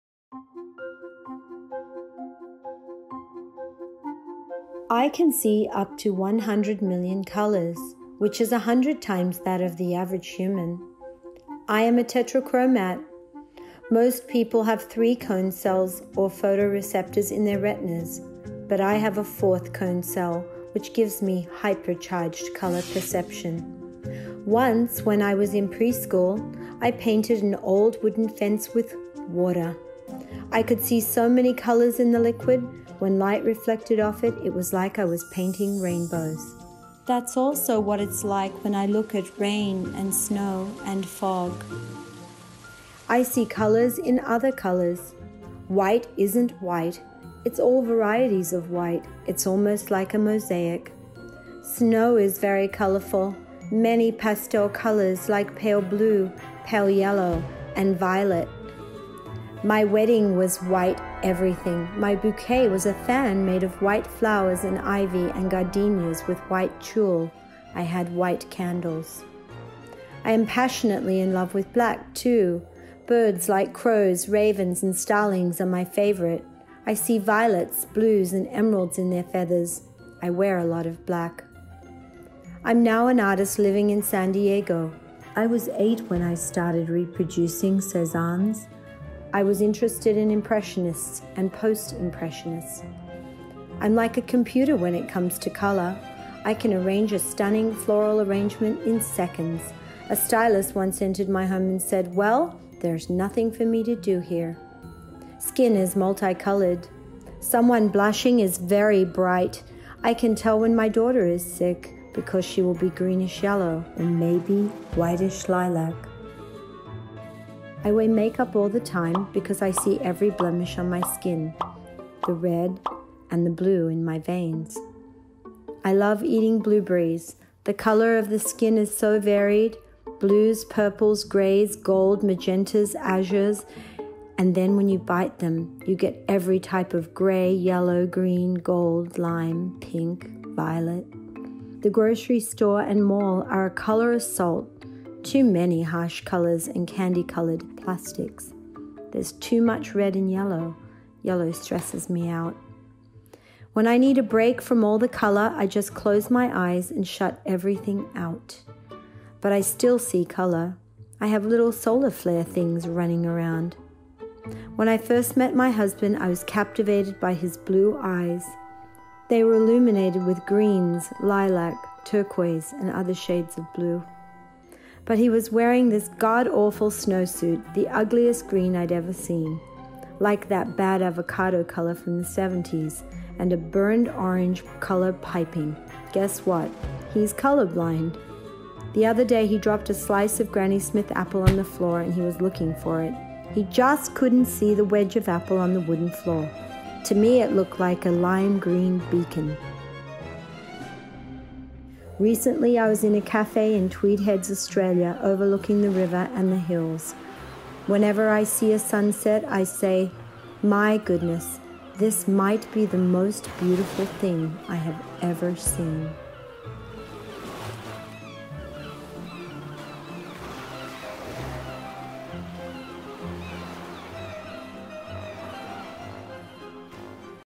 What It's Like To See 100 Million Colors
Concetta Antico is an artist with a superhuman power: she can see up to 100 million colors, a hundred times more than the average human. She is a tetrachromat: a person with four (instead of three) photoceptors in their retinas, thus possessing hyper-charged color perception.

In the video above, Antico shares with the Cut her experience living in a rainbow world of color that's inconceivable to most of us.